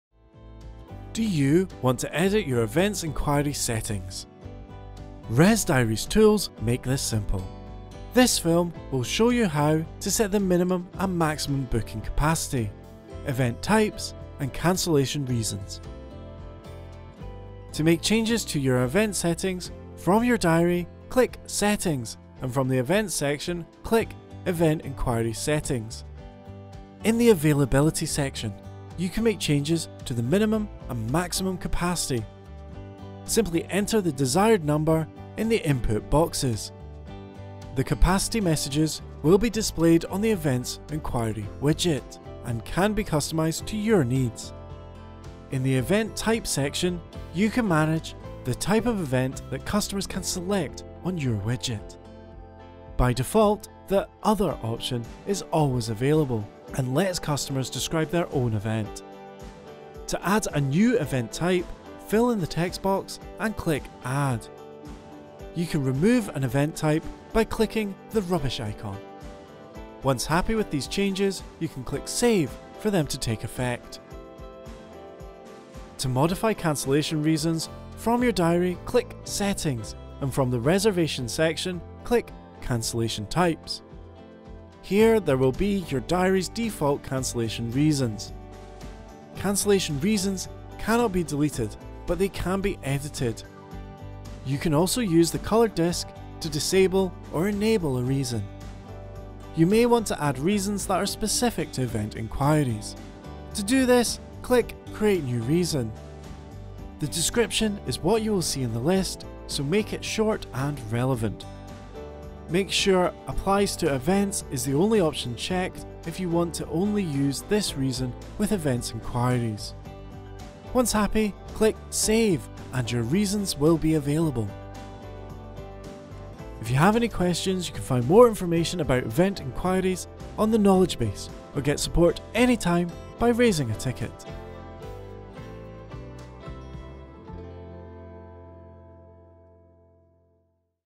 Events manager: settings
Do you want to edit your event enquiry settings? ResDiary’s tools make this simple. This film will show you how to set the minimum and maximum booking capacity, event types and cancelation reasons.